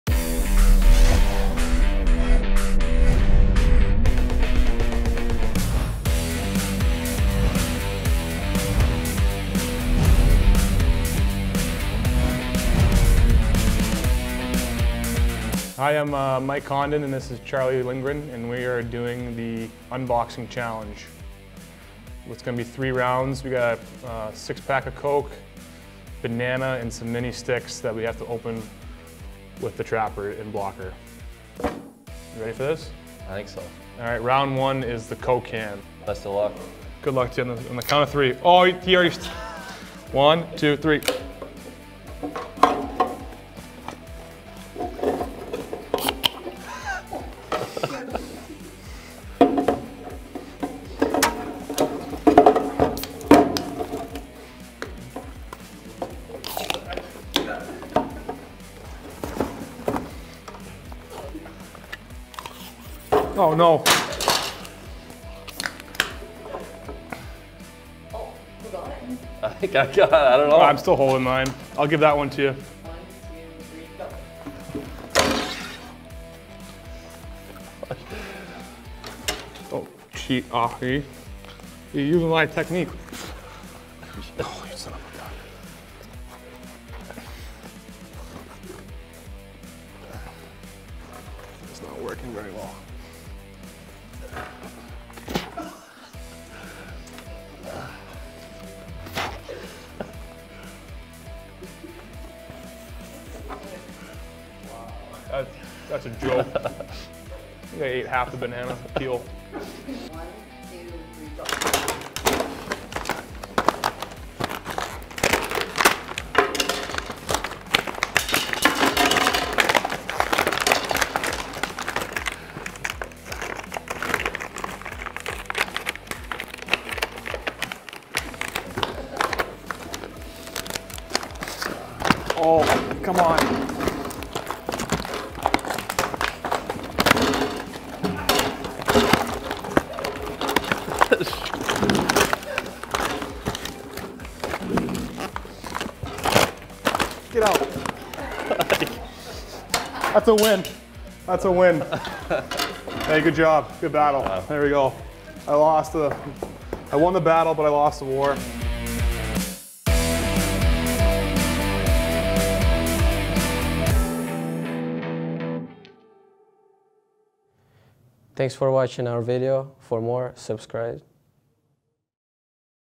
The Duel: Unboxing
Mike Condon and Charlie Lindgren wrestle with some packaging in the Duel.